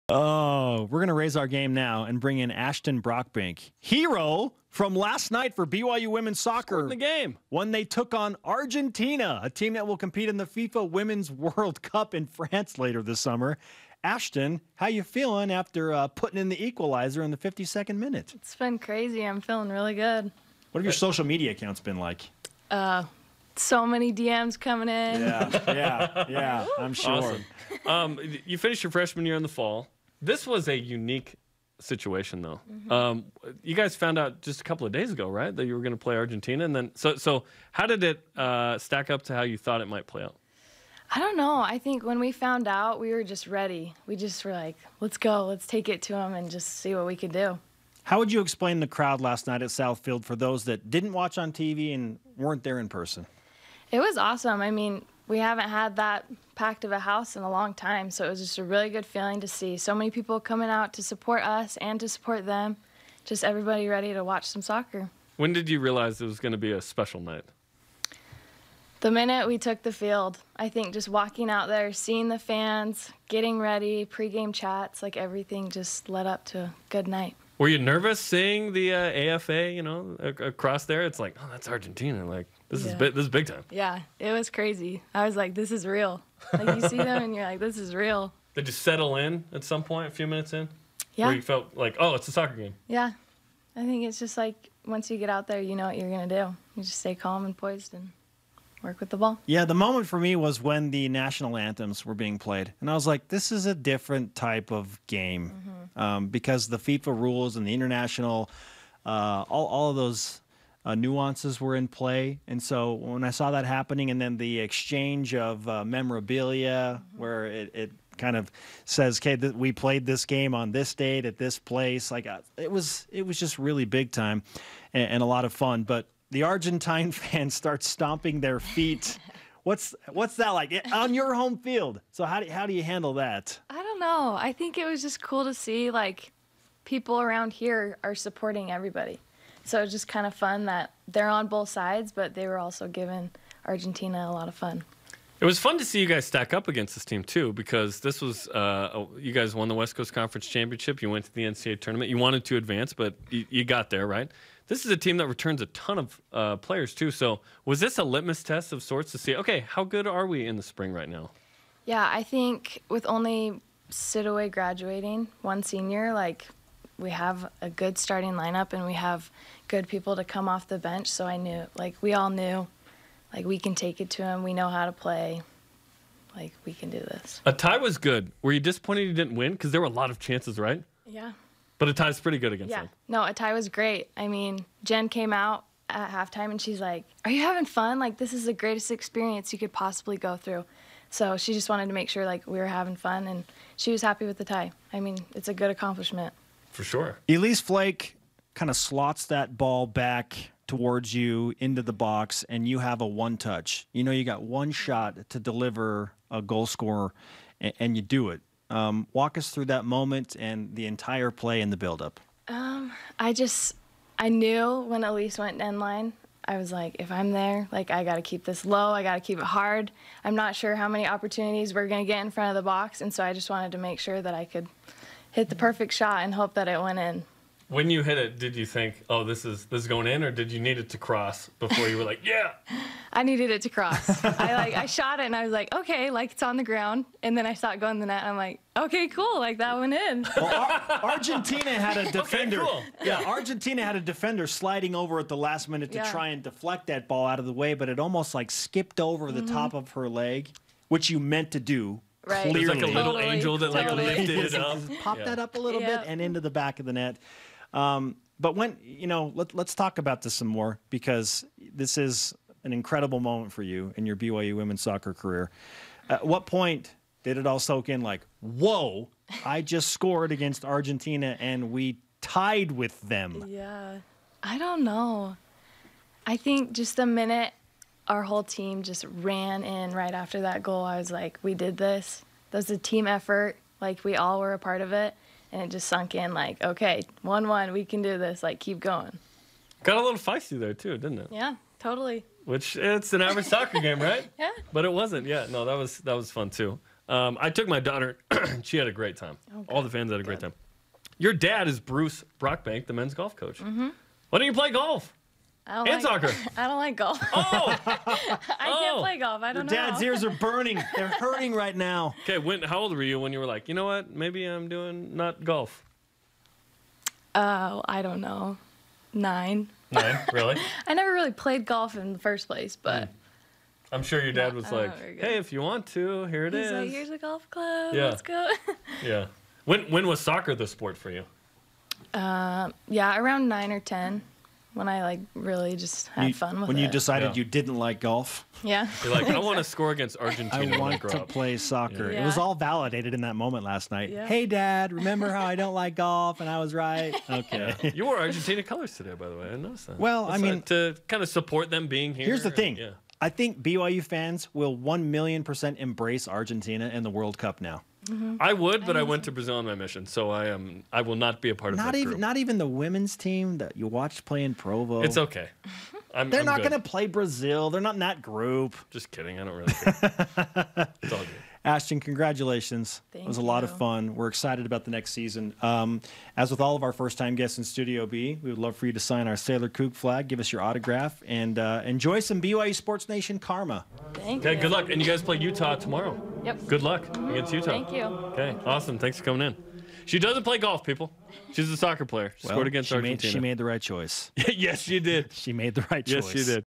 Ashton Brockbank on BYUSN 4.5.19
Freshman Ashton Brockbank scored the only goal for BYU in last night's friendly against the Argentine Women's National Team. Today she's on BYUSN to share what went into the goal and the moment she knew that this game was going to be special.